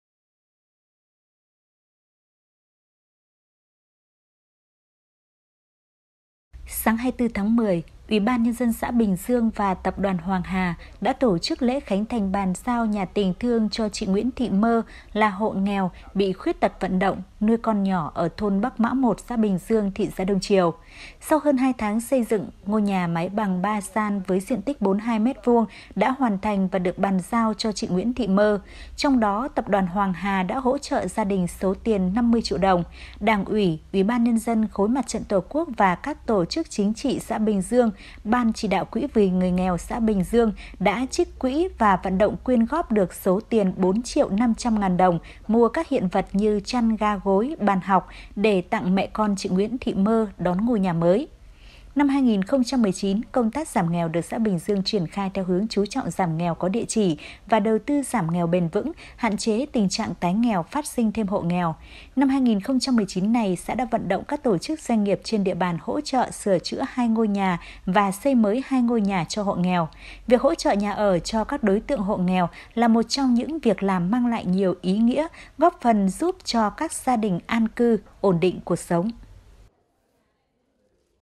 Đông Triều tv-Bàn giao nhà tình thương cho hộ nghèo ở Bình Dương
Bàn giao nhà tình thương cho hộ nghèo ở Bình Dương
(Đông Triều tv- kênh thời sự chính trị liên tục cập nhật)